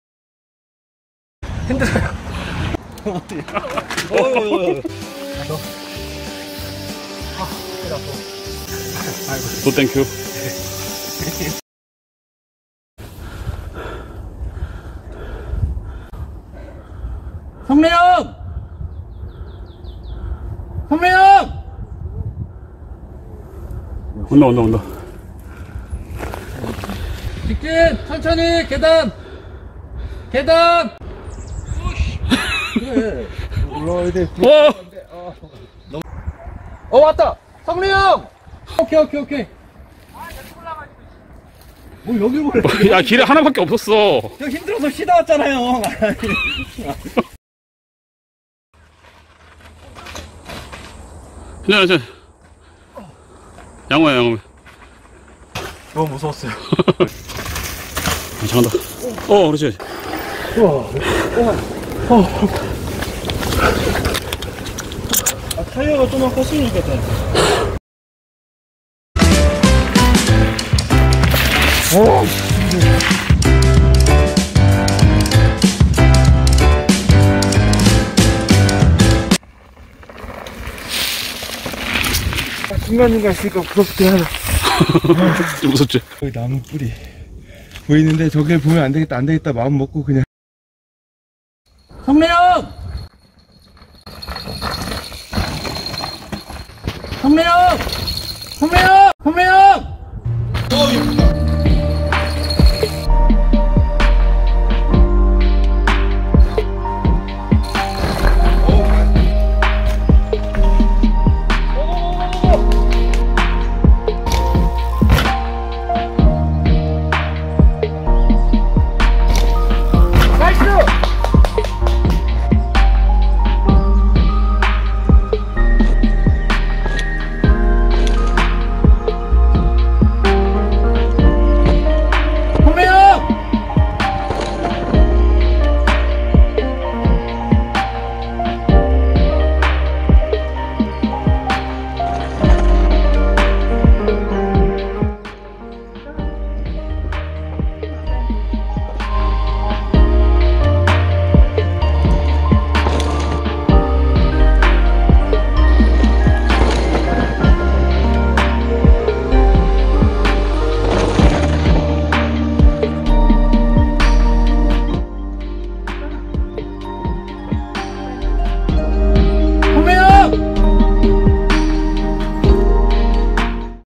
대반전!! 초보 점프 마스터│MTB 이젠 점프도 두렵지 않다
모두 한때는 초보였으나 이젠 점프 드랍 마스터
별M 초보 3 점프 도전 탈출기
멋진 삼총사의 초보 극복 스토리 영상에 담아보았습니다.
영상에는 잘 안담겼으나 성래옹의 한마디 마지막으로 남깁니다.
"짱.~! 끝났다 이제 우리는"